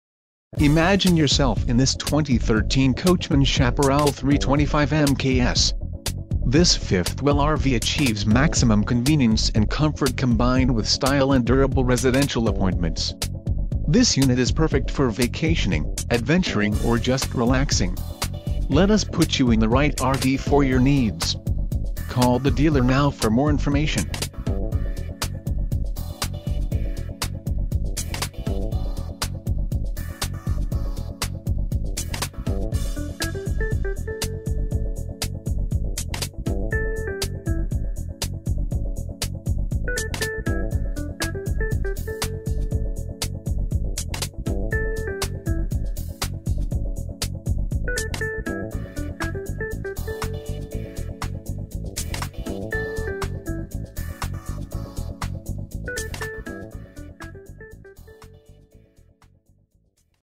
2013 Coachmen Chaparral - Colerain RV - Cincinnati, OH 4525
Excellent technology and attractive styling intersect for this new fifth wheel made by Coachmen.  you\'ll love the Nutmeg colored interior complete with three slide outs and four sleep places.